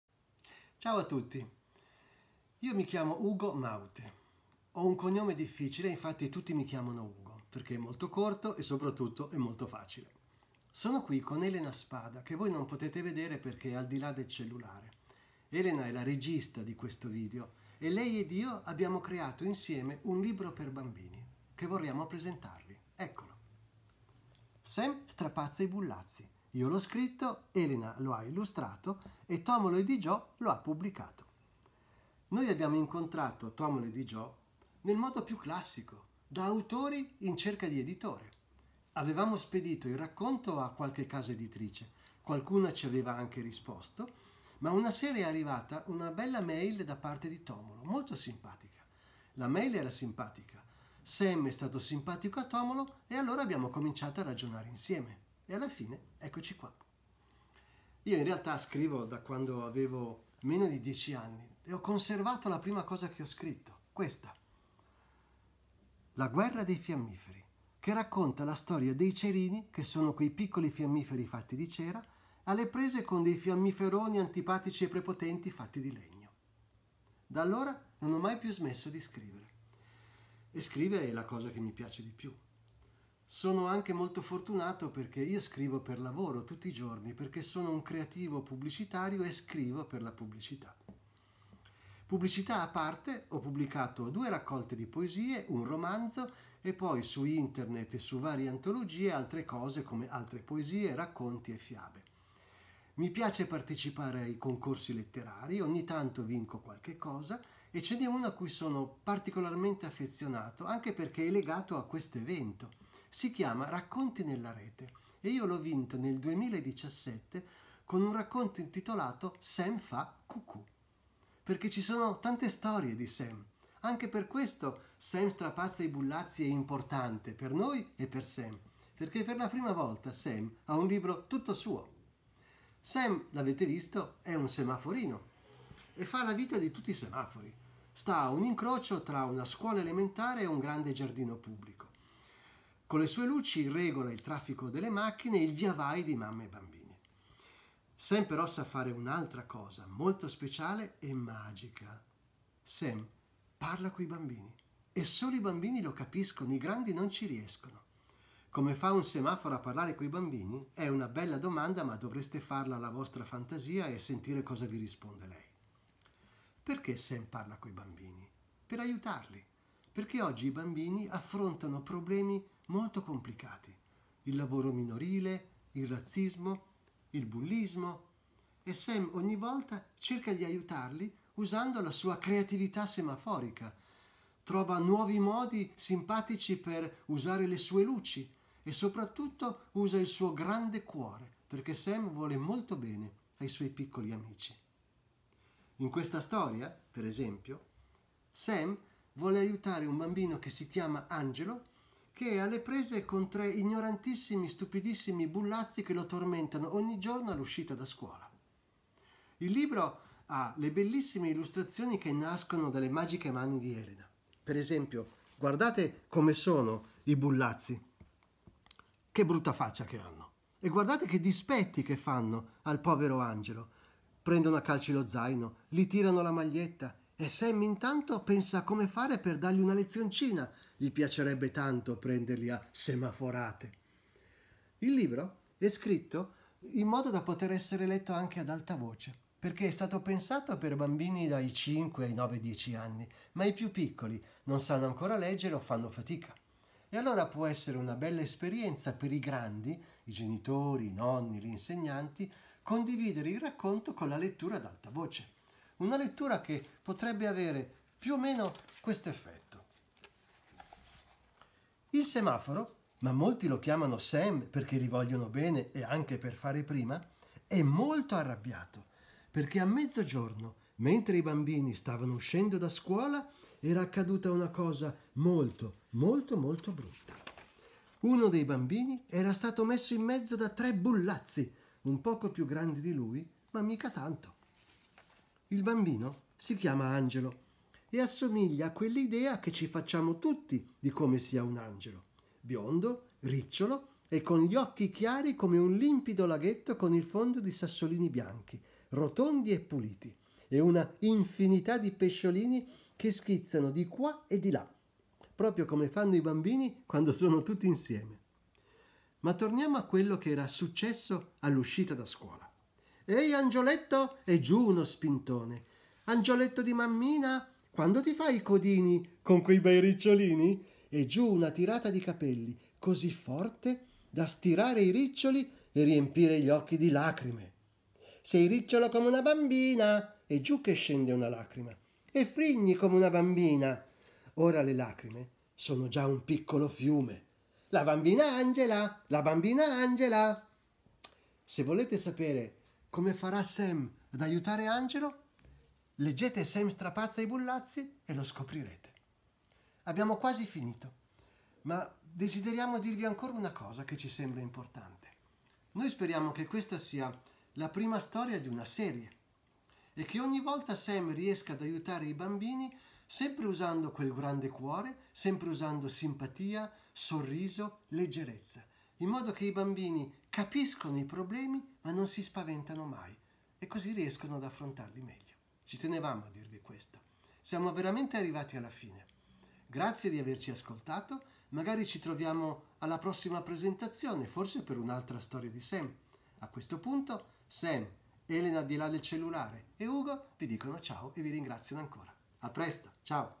SEM STRAPAZZA I BULLAZZI - UGO MAUTHE
Ugo presenta il suo libro per bambini "Sem strapazza i bullazzi", che sarà presente a Bukitaly, 20-29 Giugno 2020.